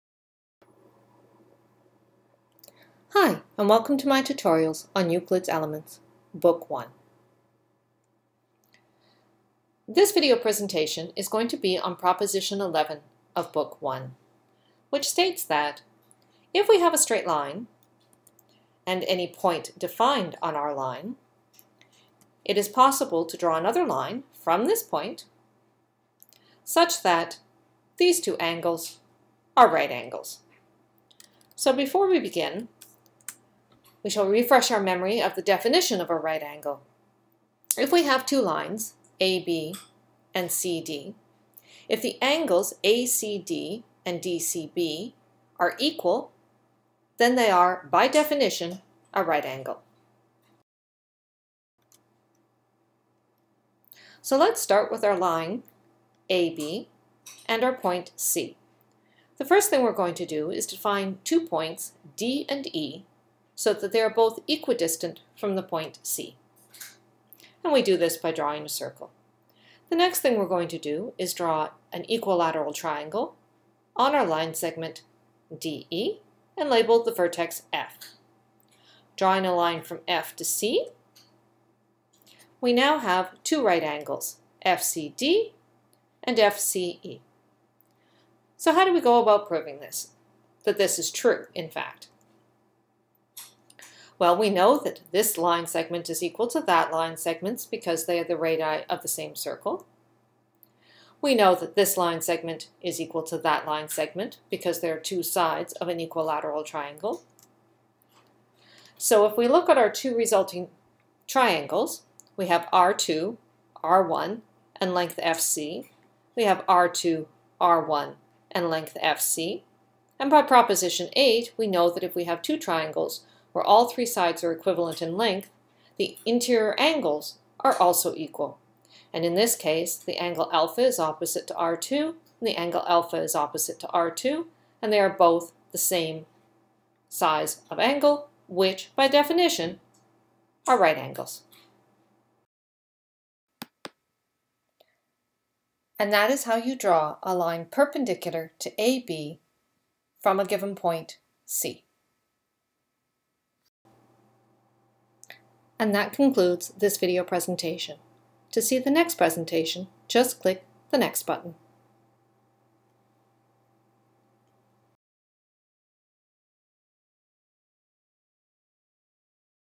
Euclid's Elements Book 1 - Proposition 11
How to draw, from a given point on a line, another line that is perpendicular to the first line.